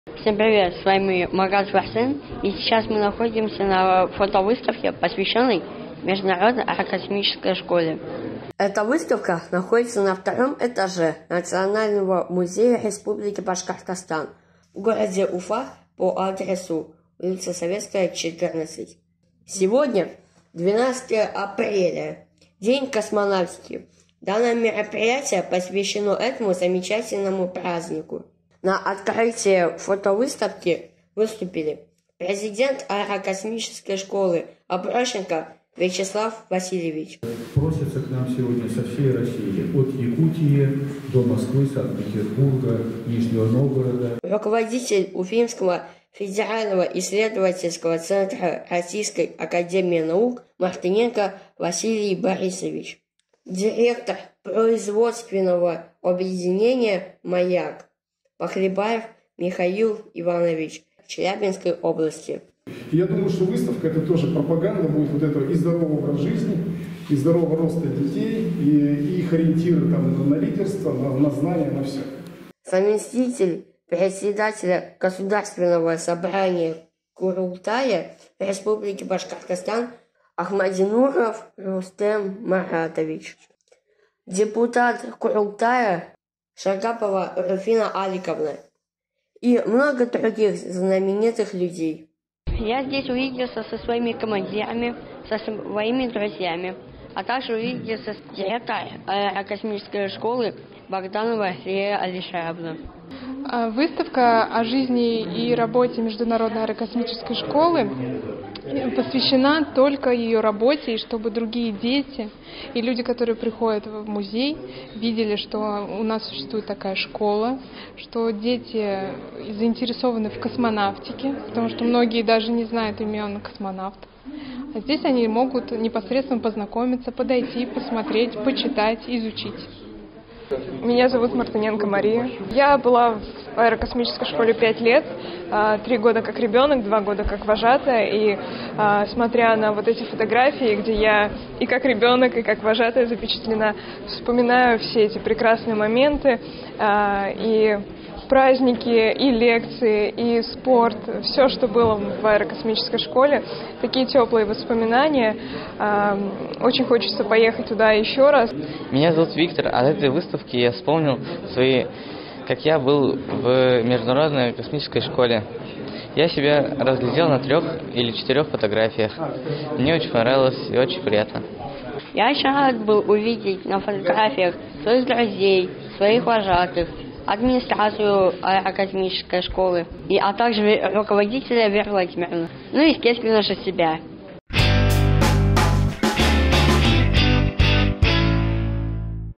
Фотовыставка
Фотовыставка о жизни и работе Международной аэрокомической школы имени космонавта-испытателя СССР У.Н.Султанова. д. Калиновки Давлекановского района